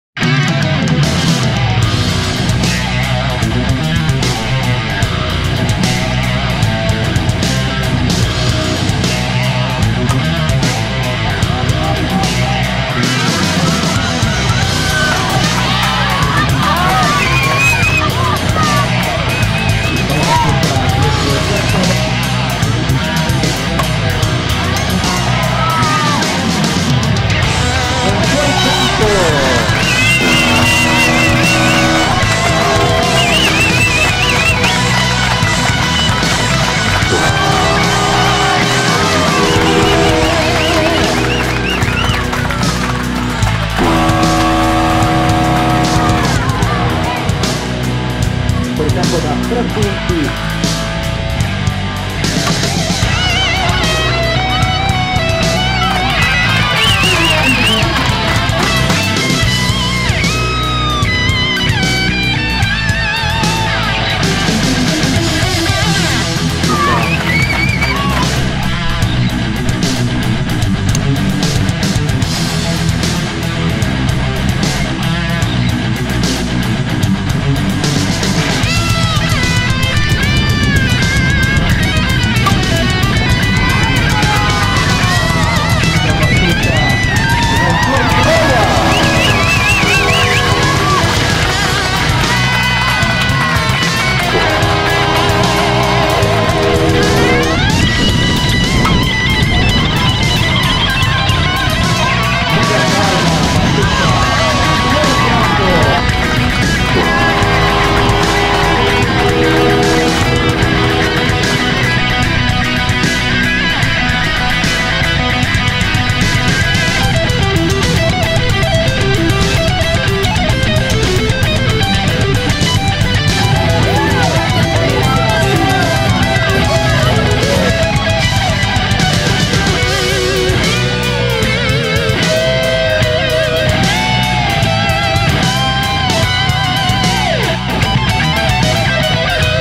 Europeo Softball 2021 - Highlights Italia vs Germania
L'Italia completa la seconda fase con un netto successo sulla Germania. Gli highlights.